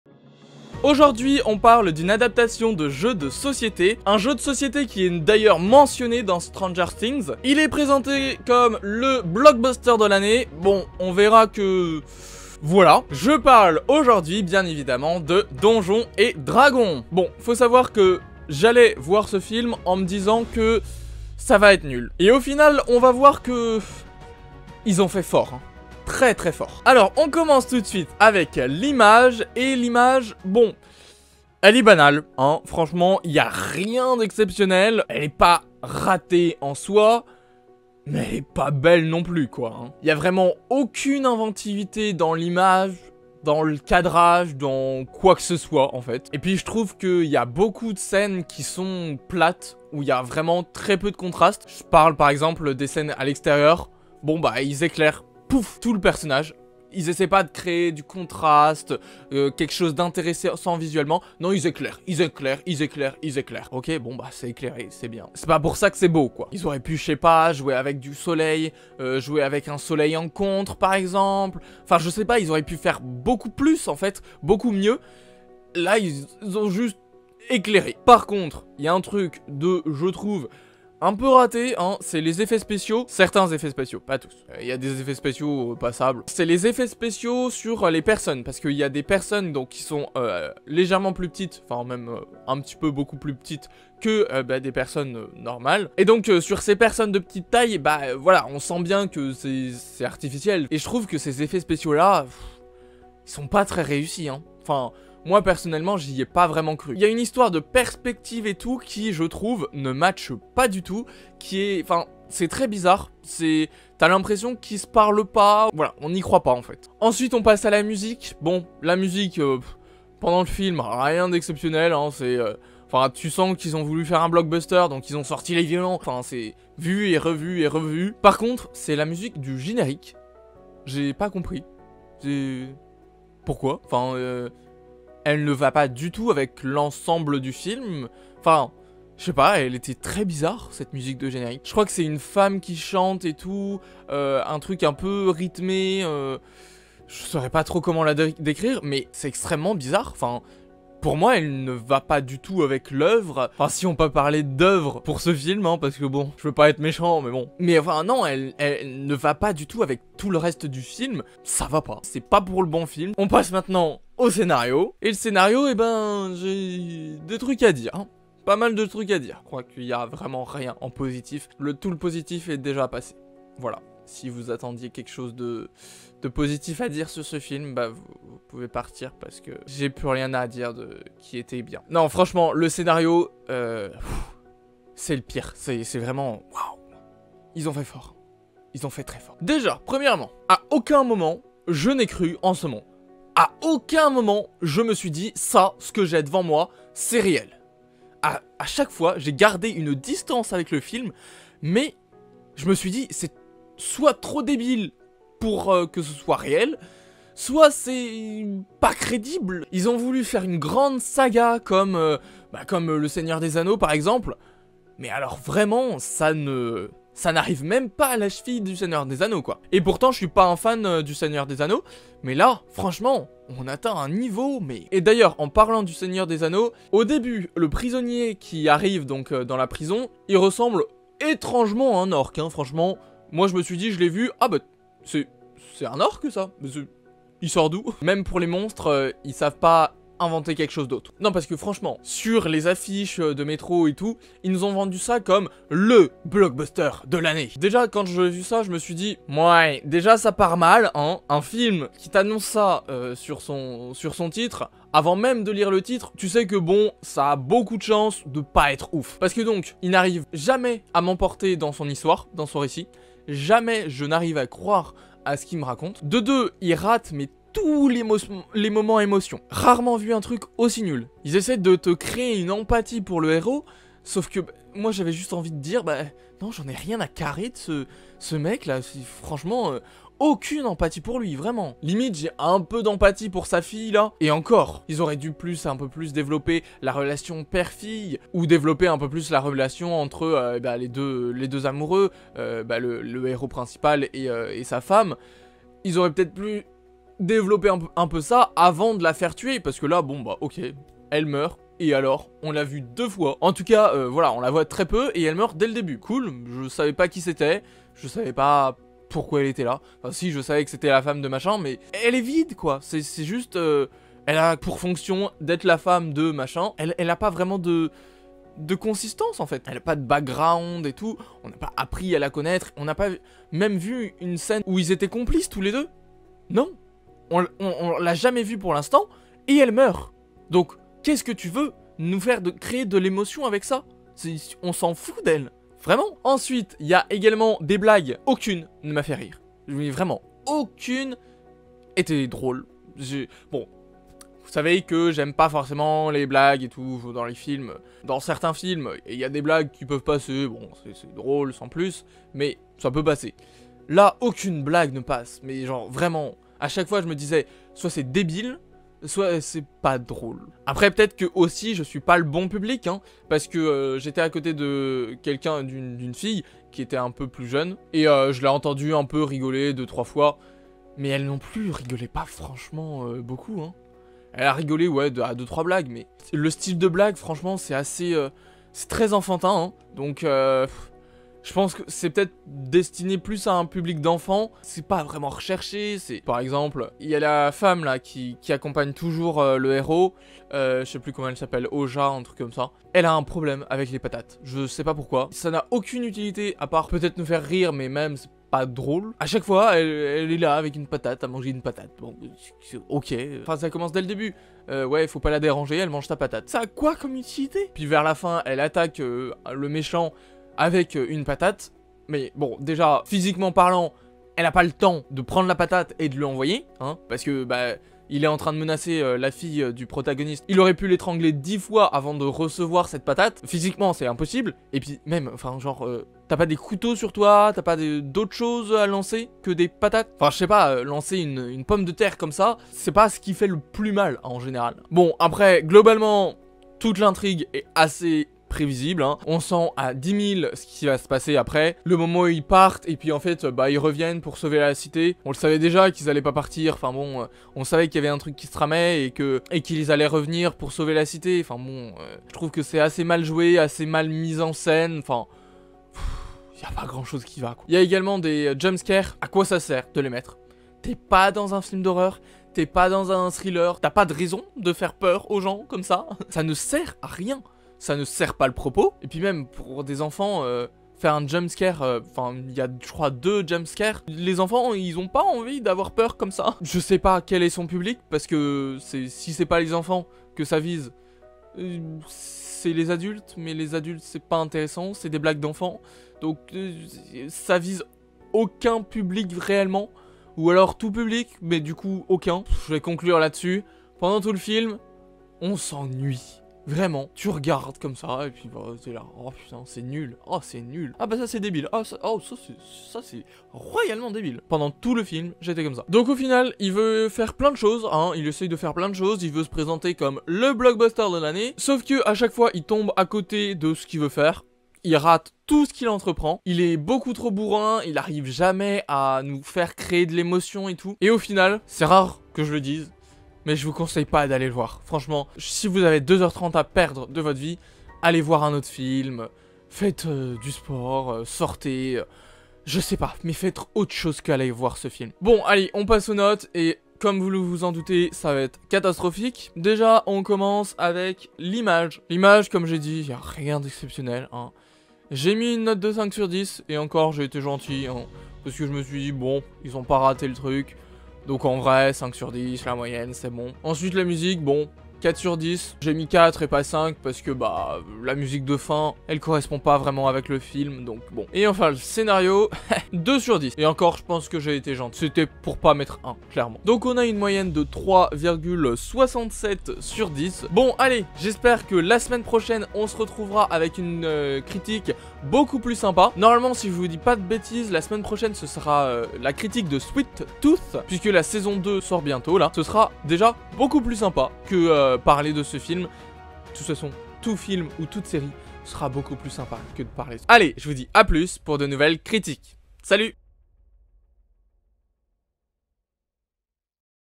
Donjons et Dragons - Critique
On parle aujourd'hui de la récente adaptation des jeux vidéos Mario.

Si tu lis cette description tu es motivé BRAVO !

Sommaire :
00:00 : Introduction
00:26 : L'Image
01:56 : La Musique
02:43 : Le Scénario
08:55 : Les Notes